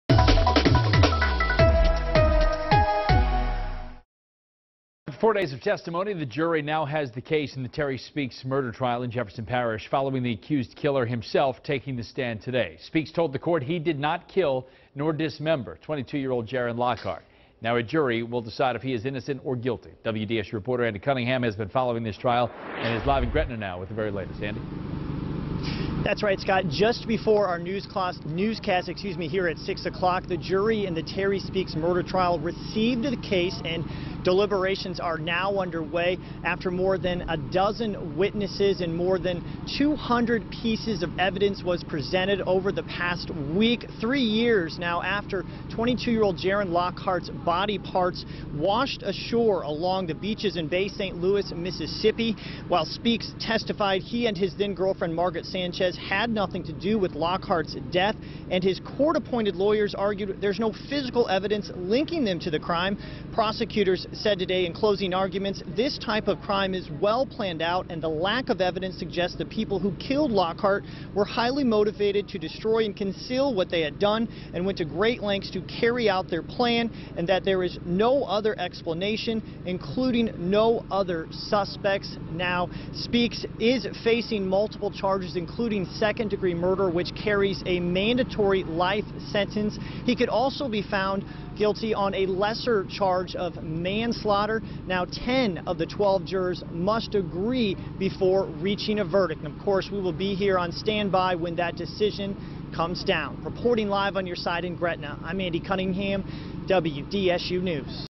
Deliberations begin in Terry Speaks murder trial
Deliberations began Friday evening in the murder trial of Terry Speaks -- one of two people accused of killing and dismembering Jaren Lockhart in 2012.